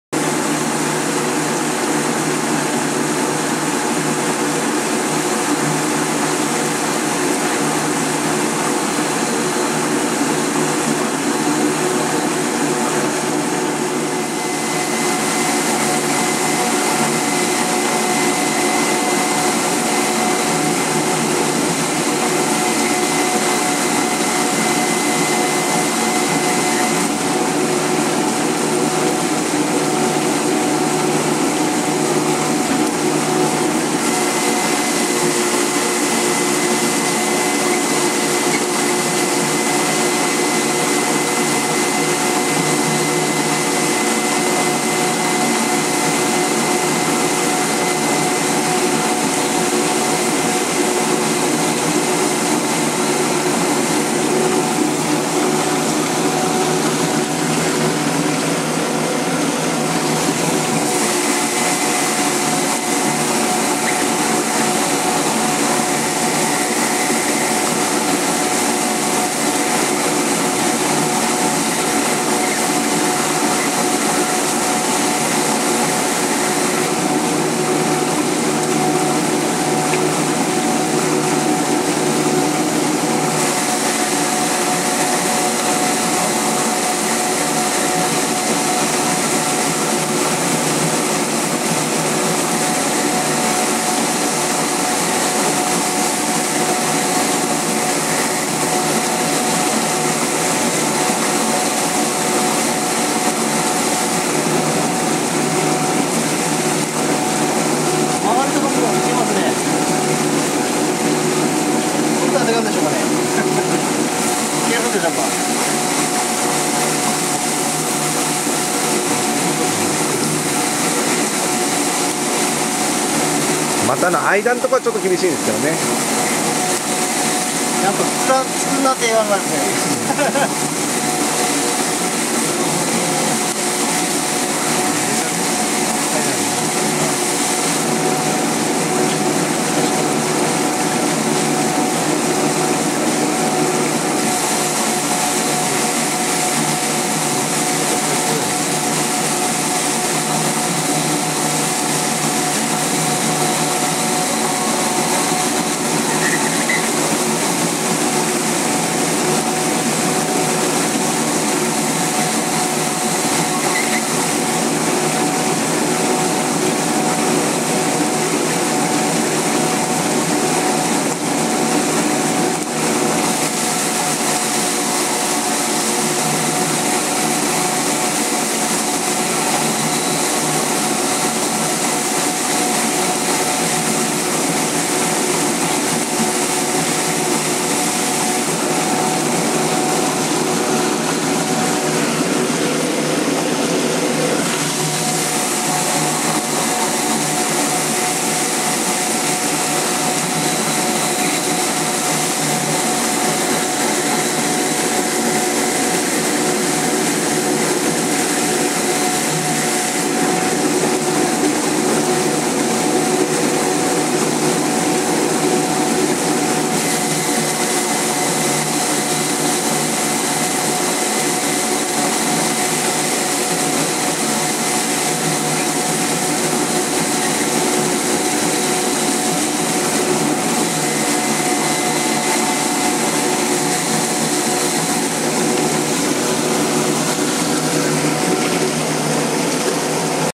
Radish Washing Machine DZ-1
Radish Washing Machine DZ-1 made in Japan.
Max efficiency: 300 radishes per hour.